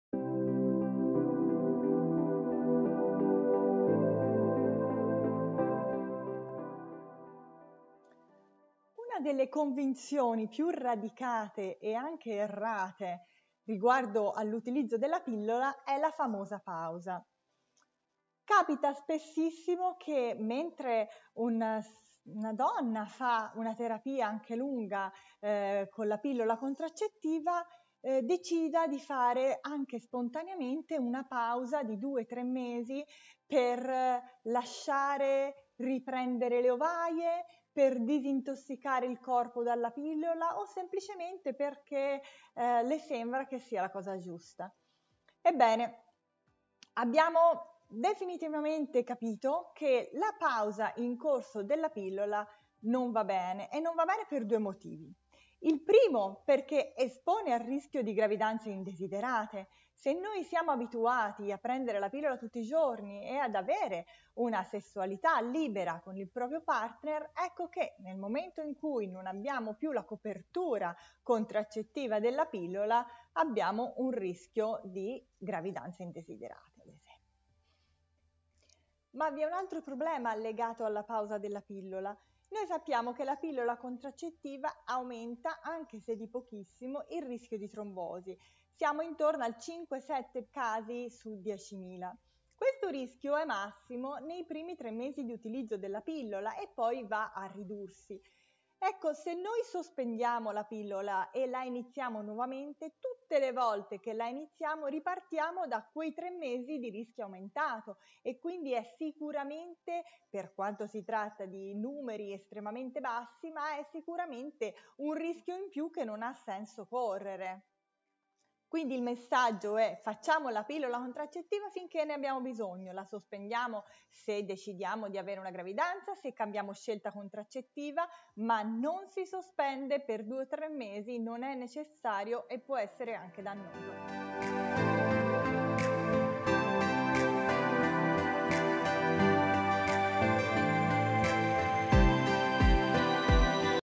Bisogna fare la pausa in corso di pillola contraccettiva?
In questo video affronterò il comune problema della pausa della pillola contraccettiva. E' infatti una abitudine consolidata, benché errata, interrompere la pillola contraccettiva per qualche mese, nella convinzione che si debba far riprendere le ovaie o depurare l'organismo. Ascoltando il brano capirai perchè fare la pausa in corso di pillola non solo è inutile ma può essere anche dannoso.